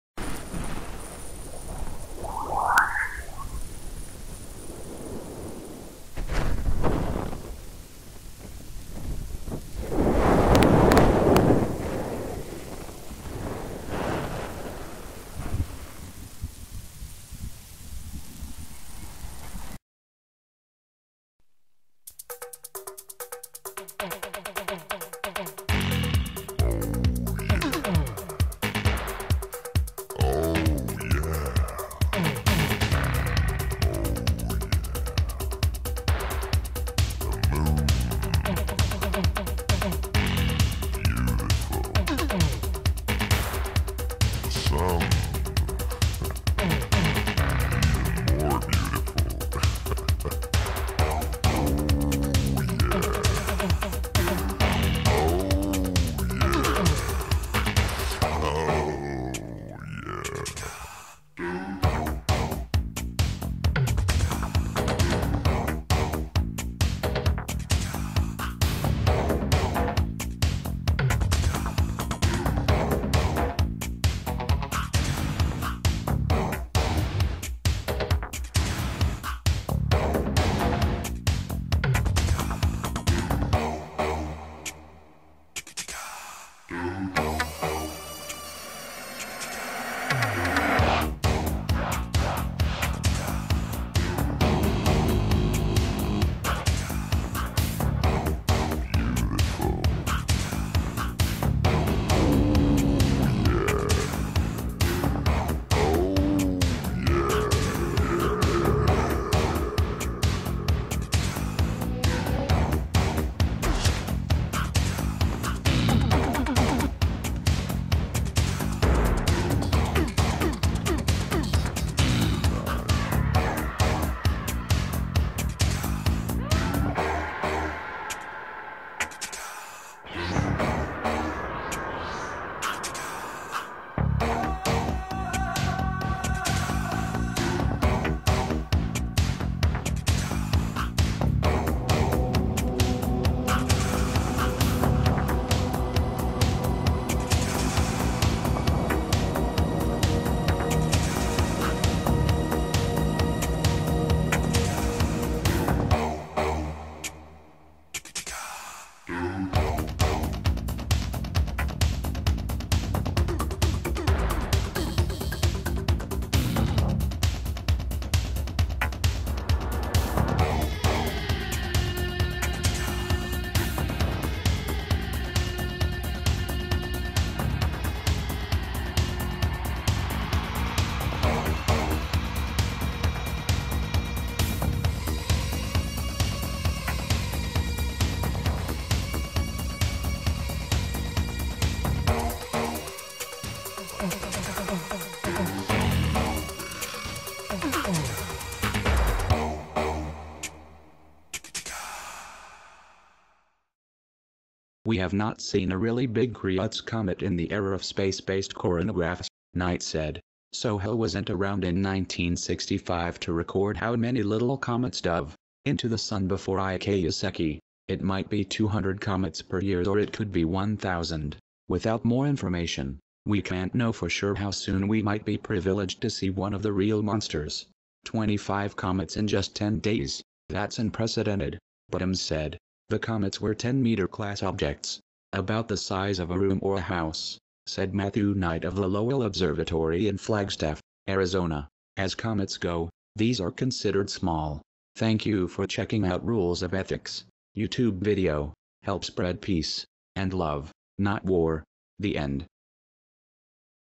WHO IS SHOOTING THE SUN MASTER (next BIG CME due to hit earth 3-11-2012)
awake and part of the solution...

"This week's solar storms have been stronger than those of recent years but moderate when viewed over the longer term.

"Most technological systems appear to be behaving well so far. A few dead batteries however, given the Sun's activity forecasters will be closely watching the geospace environment over the next few days."

The current coronal mass ejection (CME) - travelling at some 1,300km per second - began arriving at Earth on Thursday morning, after the release of two particularly strong solar flares earlier in the week.

'Wake-up call'
The next BIG CME is due to hit earth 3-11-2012 at 08:20:10AM

Activity near the Sun's surface rises and falls through an 11-year cycle it's due to peak in 2013 or 2014.